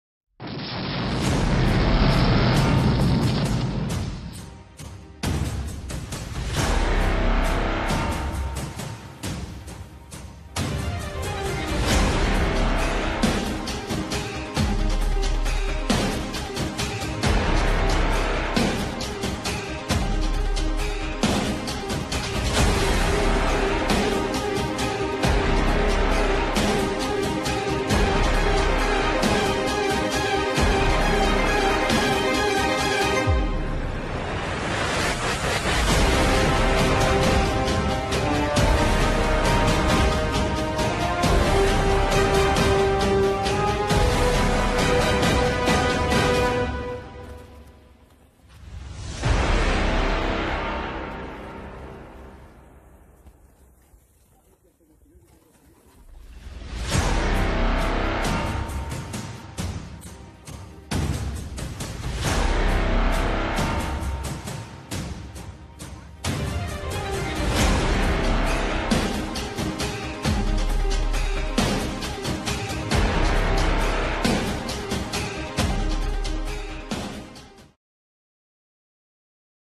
What To do in ACAPULCO?? Tehuacalco archeological site!!!! Acapulco TOUR guide...
TEHUACALCO is located 45 minutes from Acapulco, archeological site with a lot of history of the natives yopes the ceremonial place has the main pyramid and the ball game field